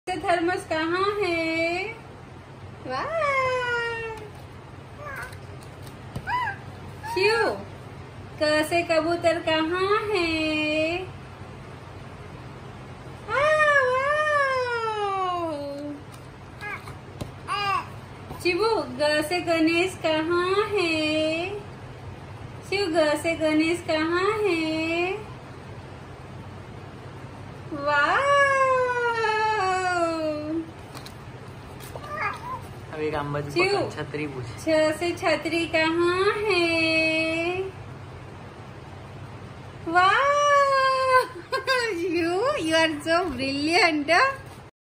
How to develop baby’s brain?? 6months old boy giving answers ??
This is brain stimulating activities done by dr Ankita Ashwini Ayurved